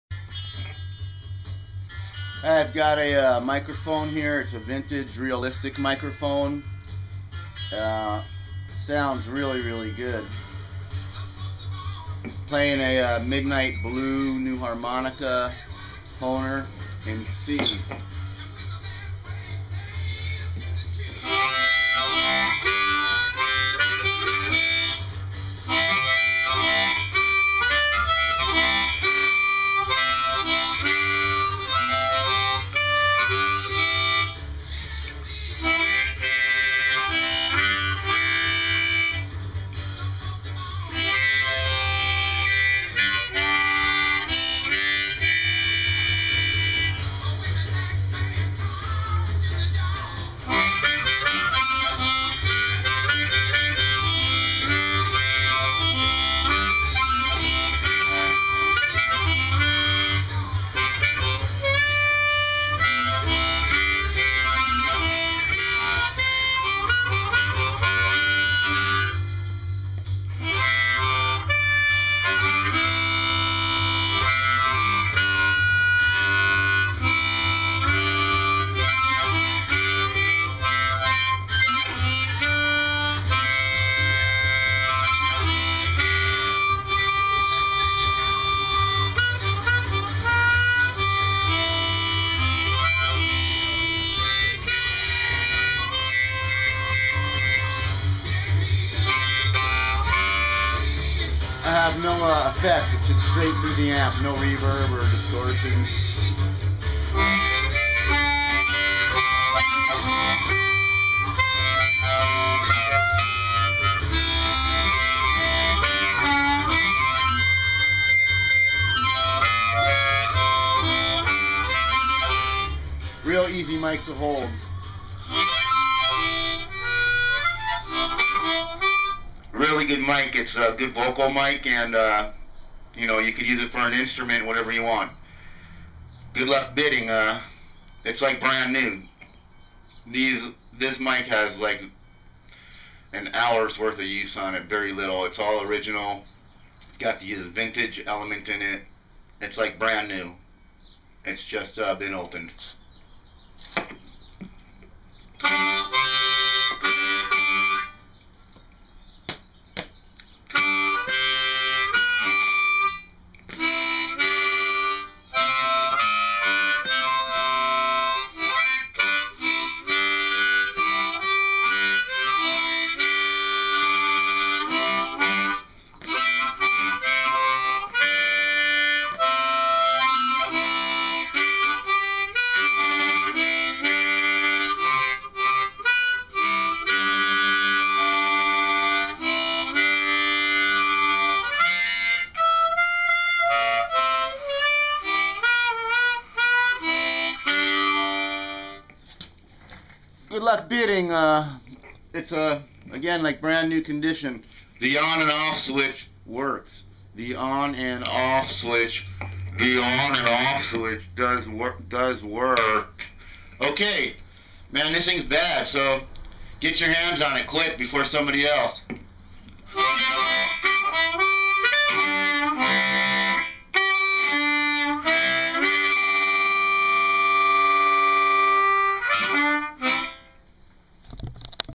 Realistic Vintage mic new open no package
For sale:  Vintage Realistic mic, sounds excellent for Harmonica, vocals, or instrument mic.  It is like new and comes with attachment to mount on mic stand and a table stand.  The grill and everything is like new and all original.  This mic is hot.